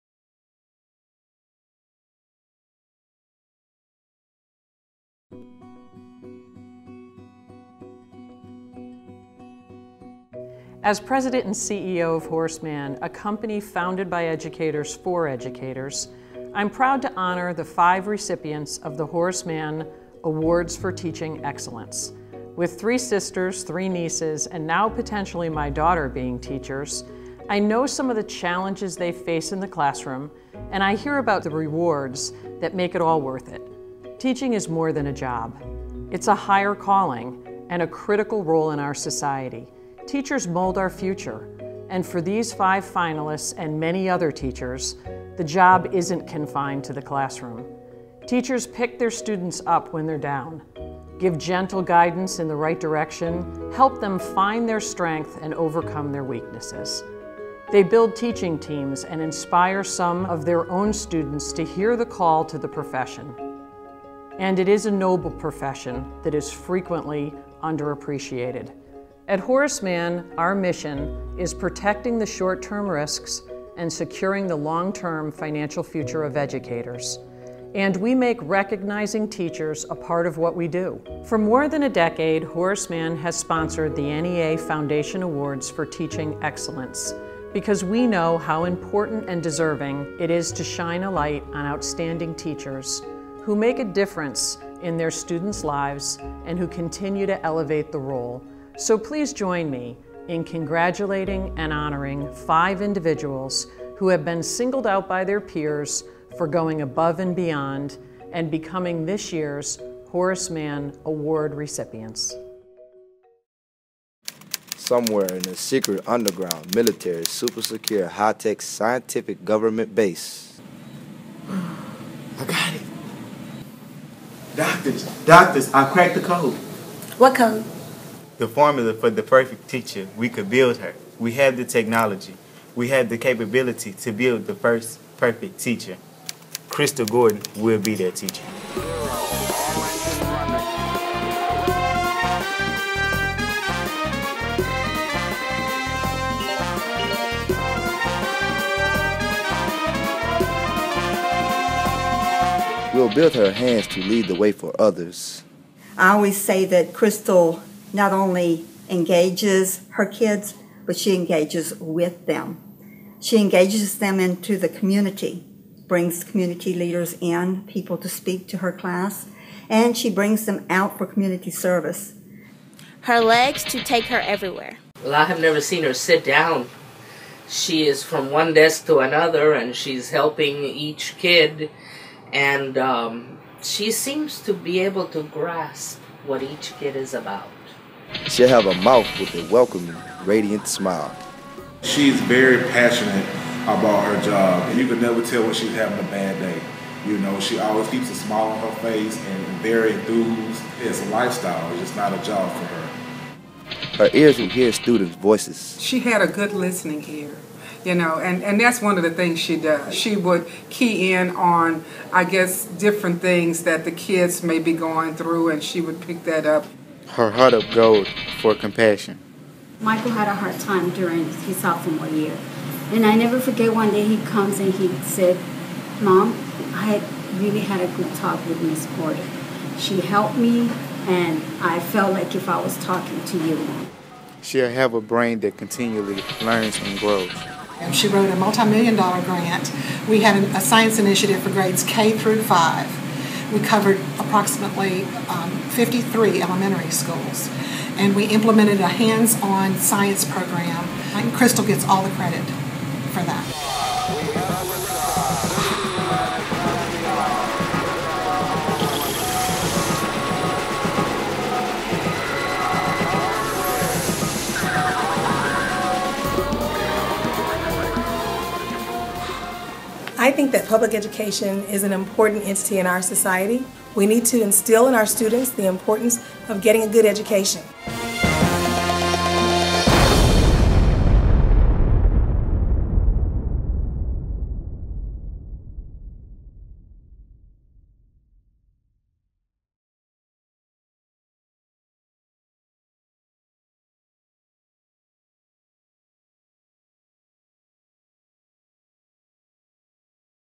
Honoring our teacher: Crystal Williams Gordon of Baton Rouge, LA
Crystal Williams Gordon received the 2015 Horace Mann Award for Teaching Excellence at the NEA Foundation's annual Salute to Excellence in Education Gala on Feb. 13, 2015. As part of the award, her students received digital arts training from Scena Media, and produced this video, which premiered at the gala, to honor their award-winning teacher.